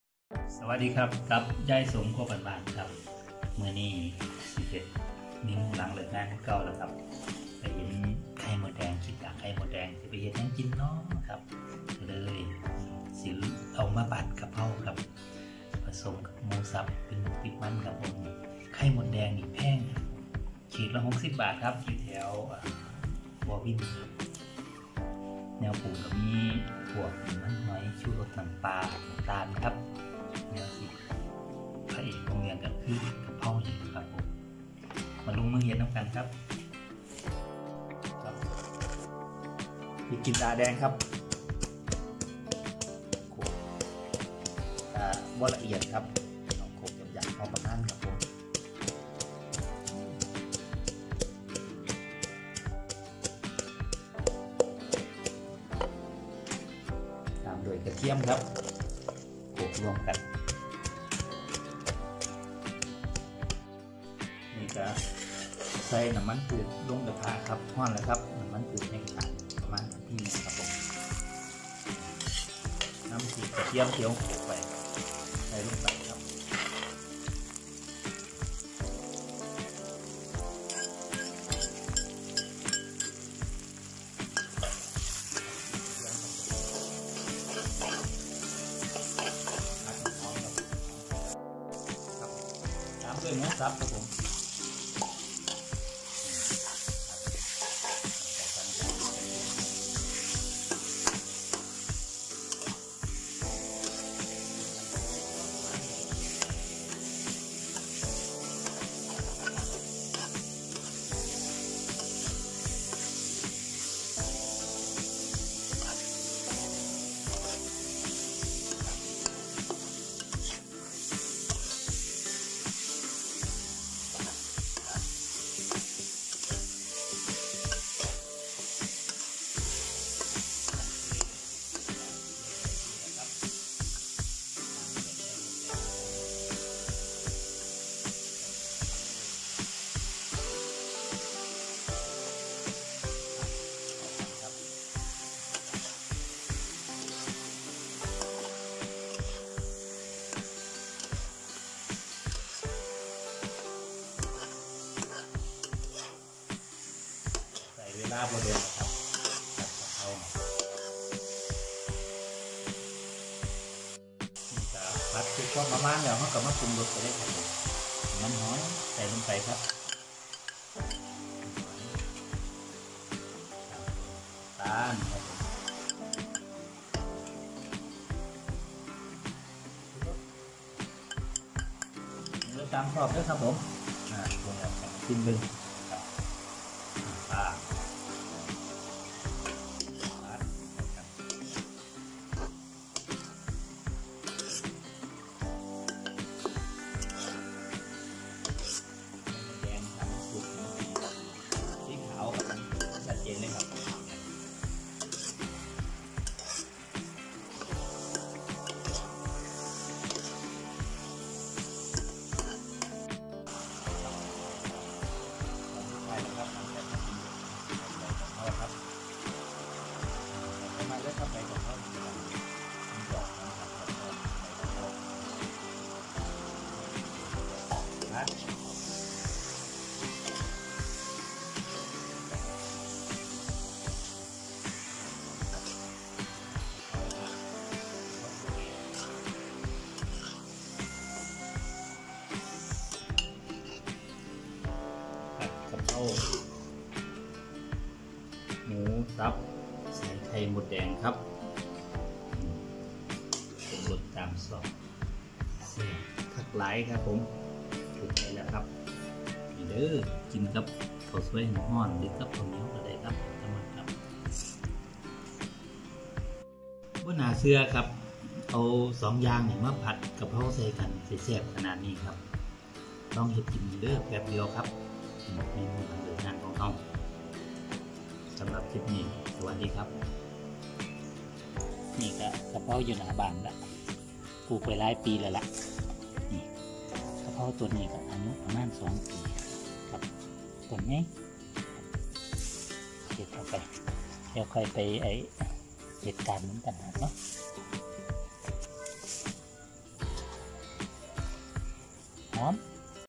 กระเพราหมูสับไข่มดแดง
#​กระเพราหมูสับไข่มดแดง​ ยายสมครัวบ้านๆ ท​ำ​อาหาร​ ไม่น่าเชื่อว่าเอาหมูสับและไข่มดแดง มาผัดรวมกันจะอร่อยมาก เพื่อนๆลองดูค่ะ